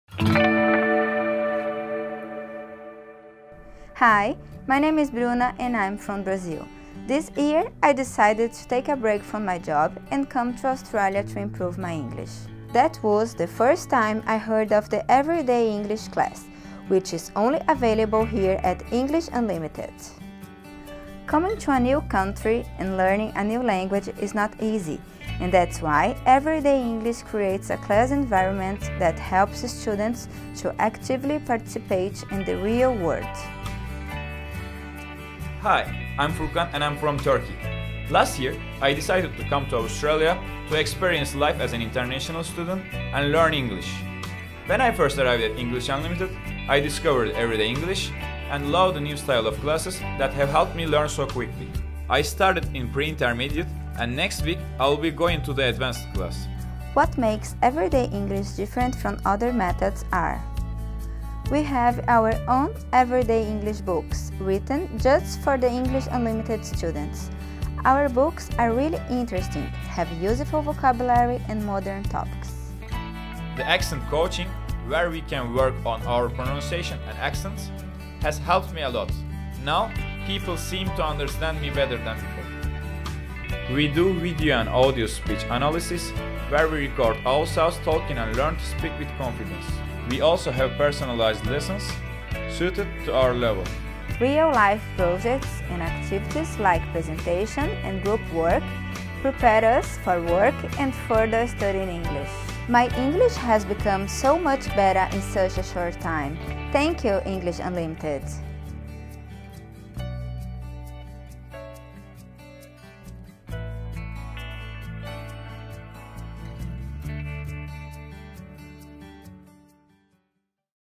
Meet Bruna & Furkan who study English in Sydney at English Unlimited!
Bruna from Brazil and Furkan from Turkey show you what it's like to study English in Sydney at English Unlimited with the Everyday English method!